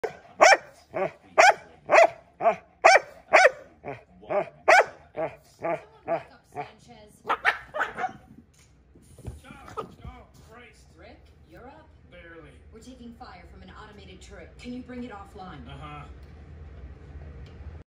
FEAR. ANGER. HATE. SUFFERING.
But that spin though!!  When Sweetie uses NFL level maneuvering tactics to obtain her one true love...food that shadow was eating first.